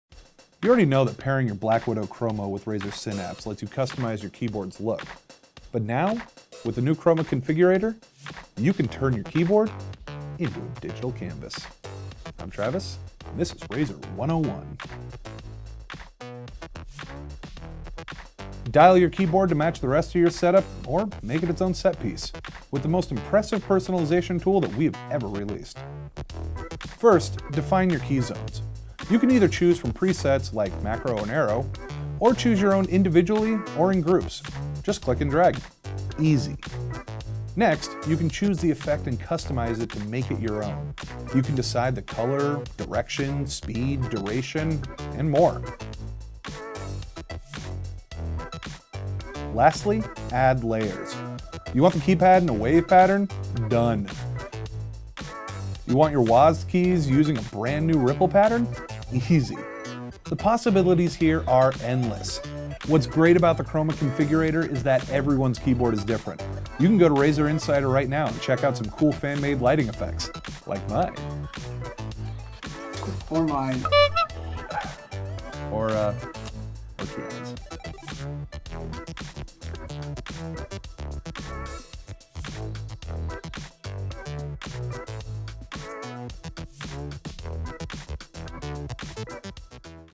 Razer Advanced Chroma Configurator | Razer 101
Travis shows you how easy it is to personalize the lighting effects on your Razer BlackWidow Chroma through the Advanced Chroma Configurator.

Head over to Razer Insider and check out more interesting effects created by the community